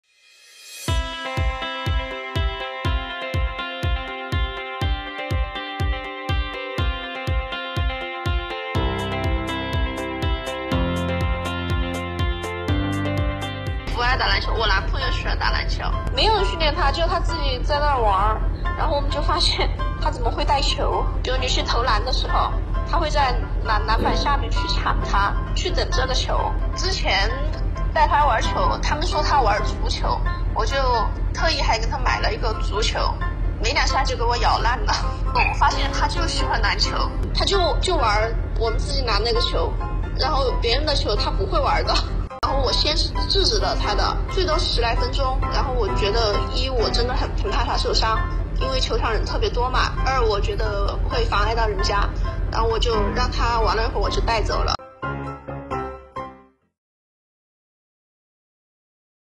Three-year-old Corgi dog in Dazhou steals limelight for passing and tackling basketball
A dog star! A three-year-old Corgi dog in Dazhou, Sichuan stole the limelight for passing and tackling a basketball on Dec. 19. Its owner says that the dog didn’t undergo training and preferred basketball to football.